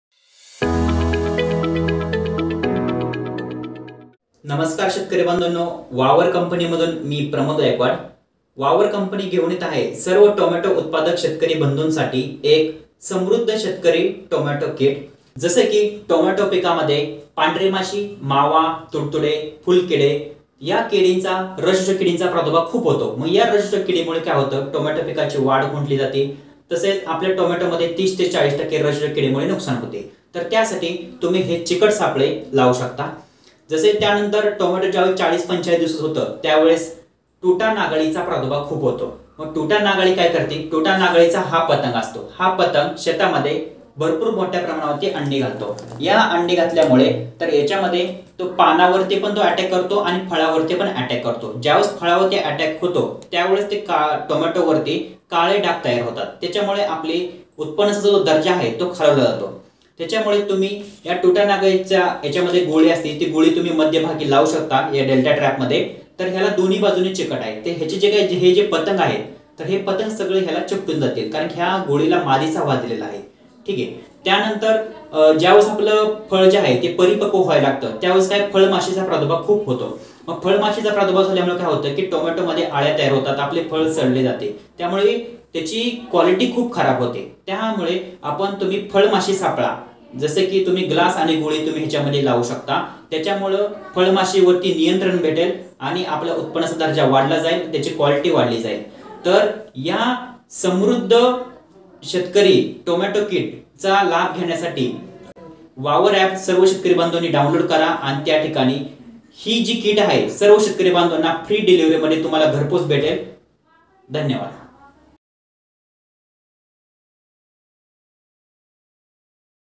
टोमॅटोमधील सर्व किडींचे समाधान बस एक किट में | Tomato leaf minor control measure | best Solution
टोमॅटो पिकांमधील मधील मावा, फुलकिडे, टूटा नागअळी, फळमाशी यासारख्या किडींचे नियंत्रण करण्यास समृद्ध शेतकरी किट- टोमॅटो किट एक उत्तम उपाय!

👉🏻ट्रैप खरेदी करने के लिए लिंक पर क्लिक करे